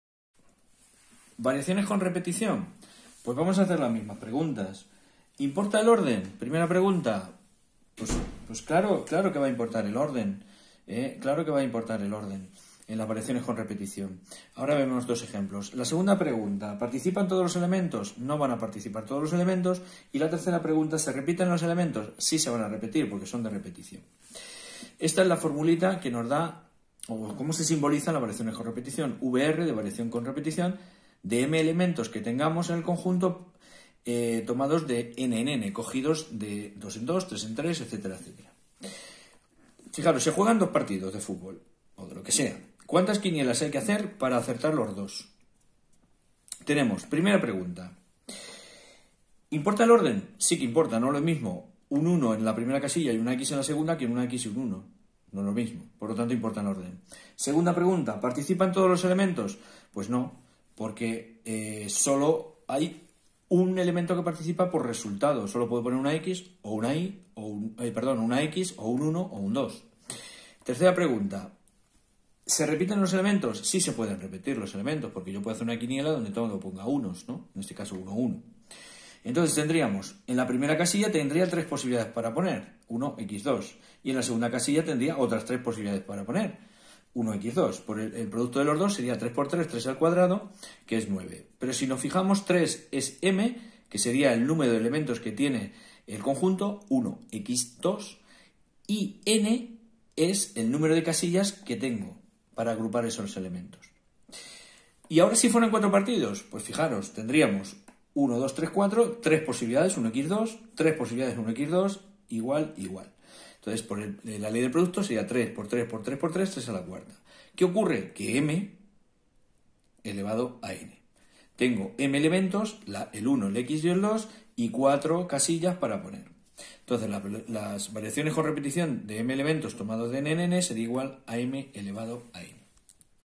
VARIACIONES con 💙 REPETICION ¡¡ejemplos!!
Las variaciones con repetición necesita de su fórmula para saber realizar ejemplos o hacer ejemplos con la calculadora.
Desde ahtria-spain te ayudamos a entender las variaciones con repetición.

Si lo que te he proporcionado te ha gustado puedes seguirme:

Blog: en construcción. Perdonar las molestias